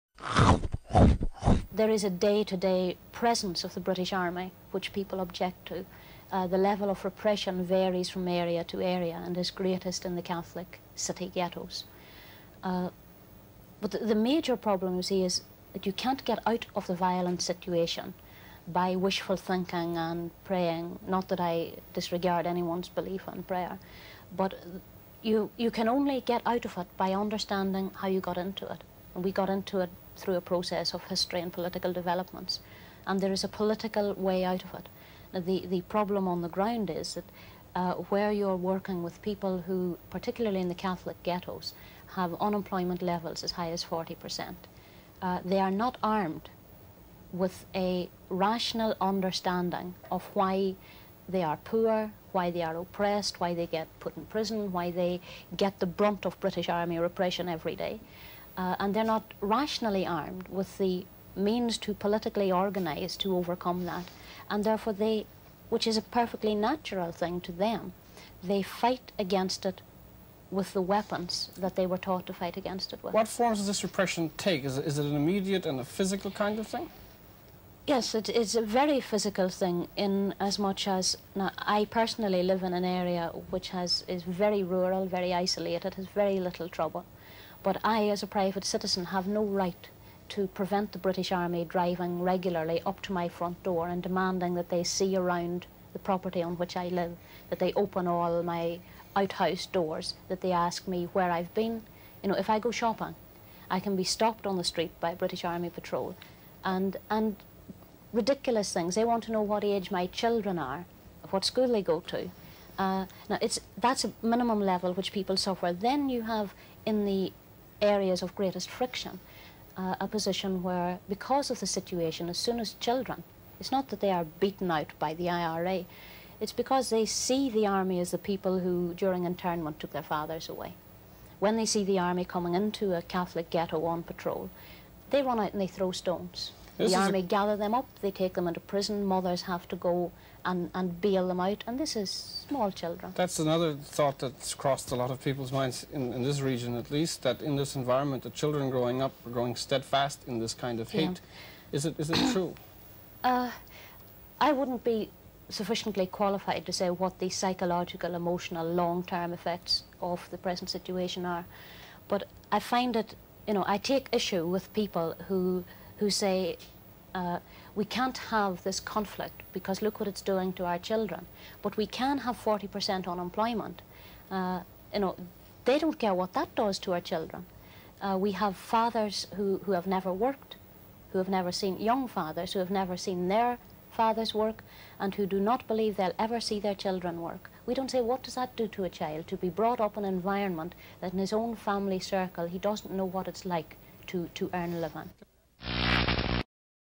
Bernadette Devlin's fight for peace in Northern Ireland: CBC Archives
In this clip from 1977, Irish political activist Bernadette Devlin talks about standing up to the British and fighting for peace in Northern Ireland. While serving as a MP in the British parliament, Devlin famously slapped Home Secretary Reginald Maudling over his comments about 'Bloody Sunday'. On that day in 1972, 26 civil rights protesters and bystanders were shot by the British Army.